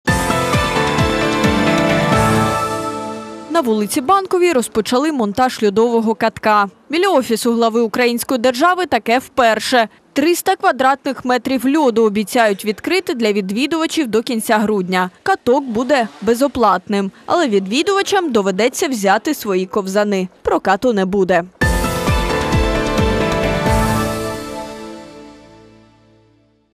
Біля Офісу президента розпочали монтаж льодової ковзанки
Покататися на ковзанах під вікнами у президента. На вулиці Банковій розпочали монтаж льодового катка. Біля офісу глави української держави таке вперше. 300 квадратних метрів криги обіцяють відкрити для відвідувачів до кінця грудня.